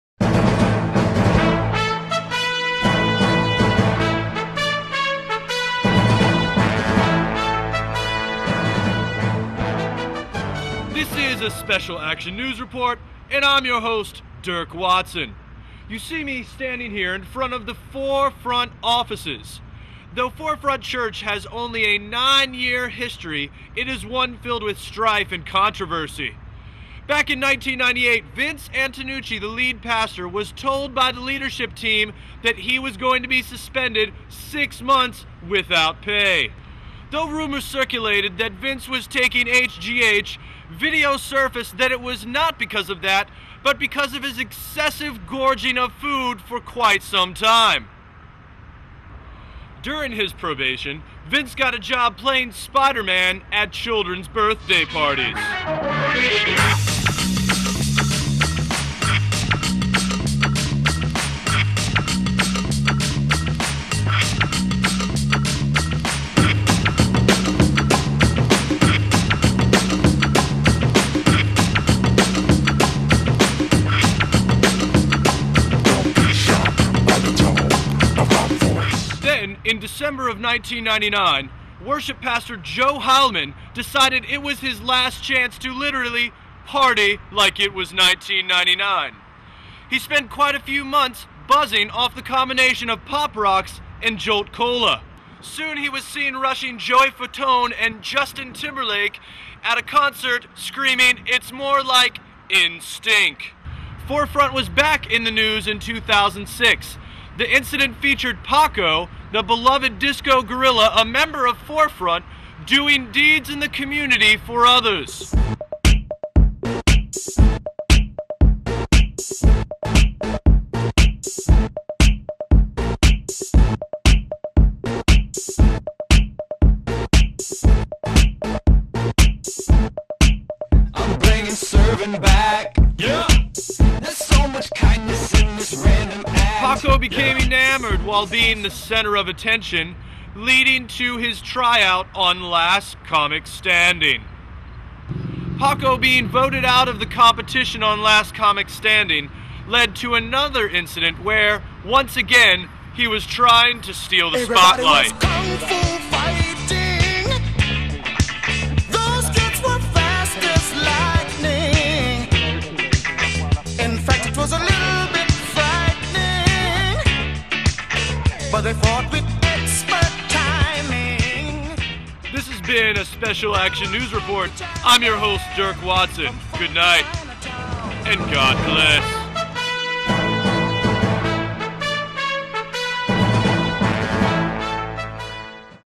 Forefront Scandals
A Special Action News Report about scandals that have happened in the news about Forefront Church